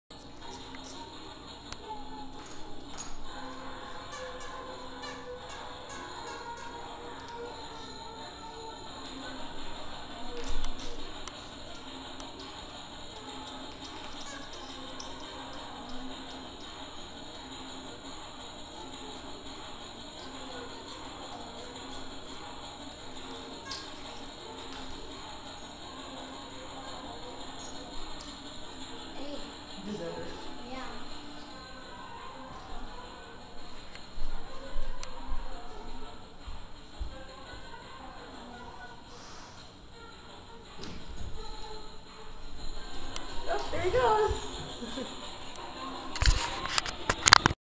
tank and his chicken.MOV
My malamute, Tank, eating his toy chicken :)